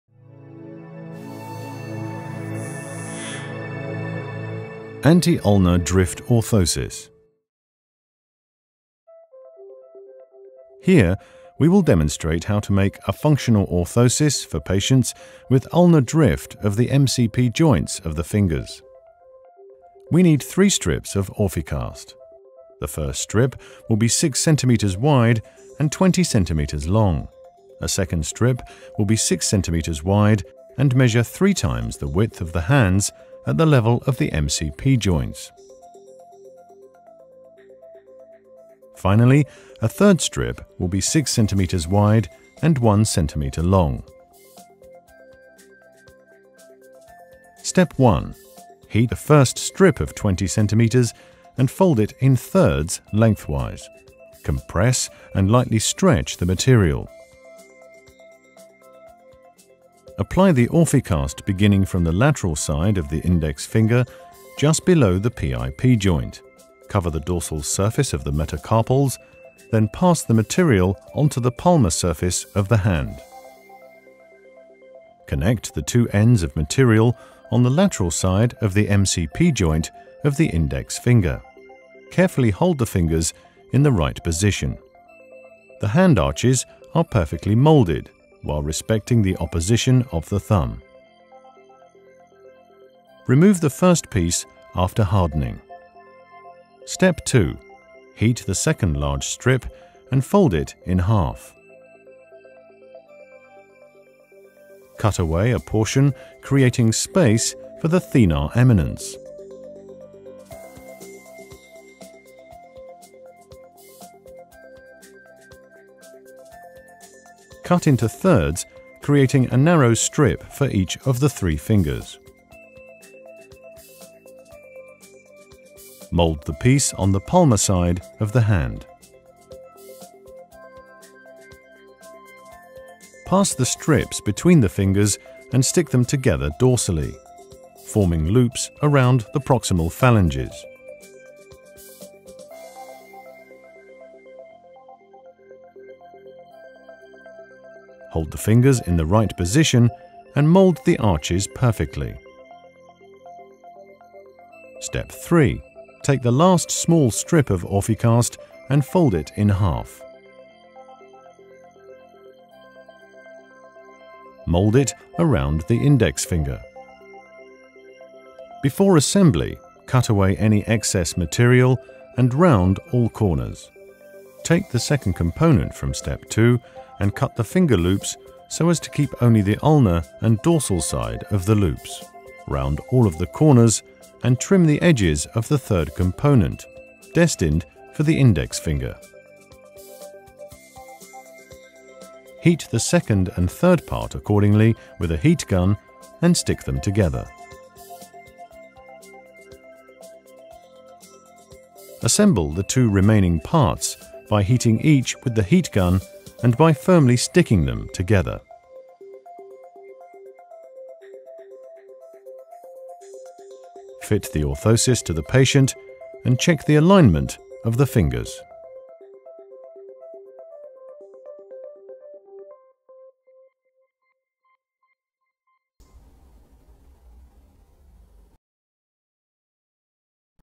Anti-ulnar drift orthosis - Orficast Instructional Movie 20
This video demonstrates how to make a functional orthosis for patients with ulnar drift of the MCP joints of the fingers.